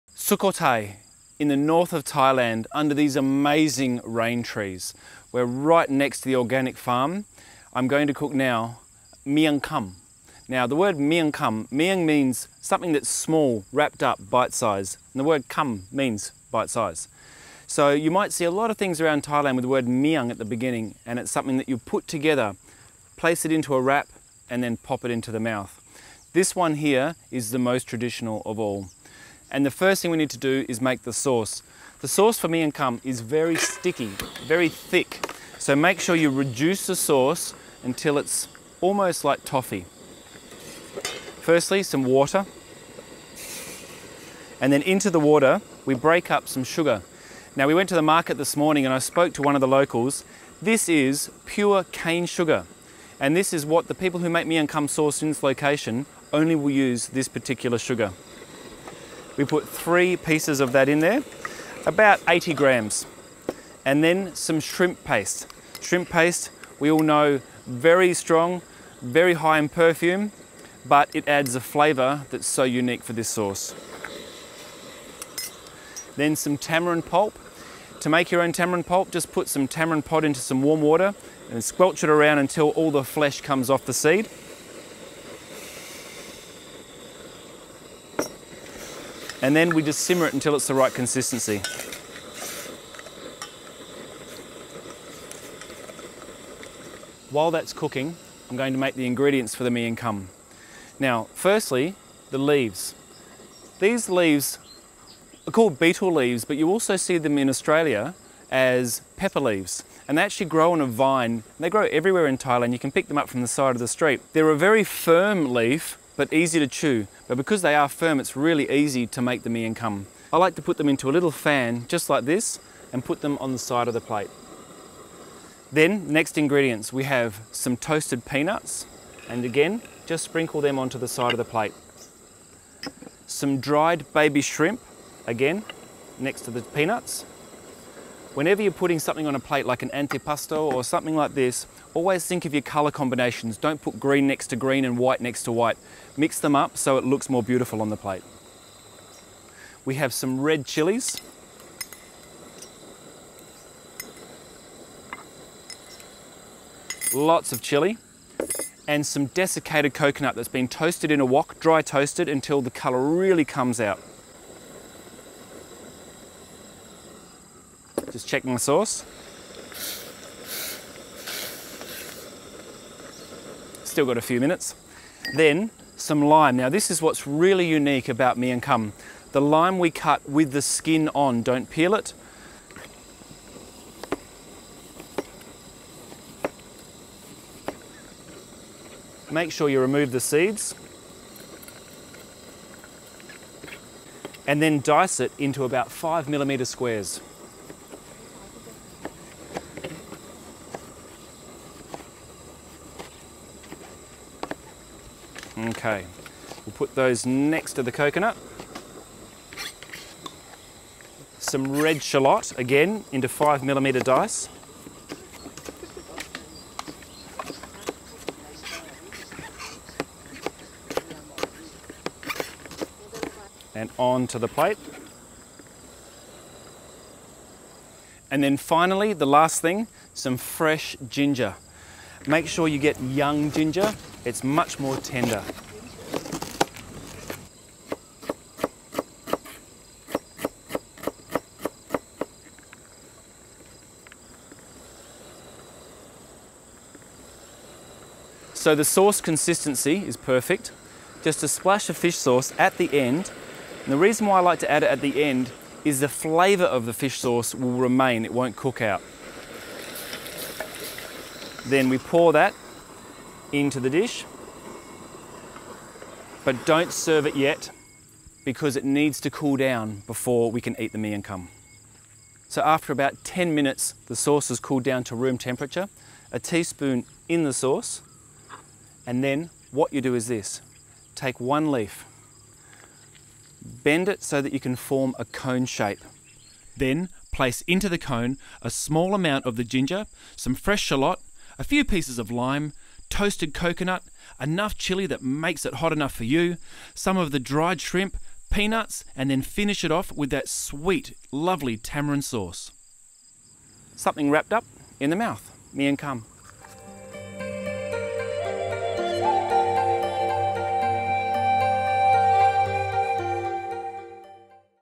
ฝรั่งทำอาหารไทย, Thai Food Miang Khum
Really interesting dish filmed on location in Sukhothai Northern Thailand. Imagine serving this one up at your next get together, easy to make and the flavours are intense as is the colour.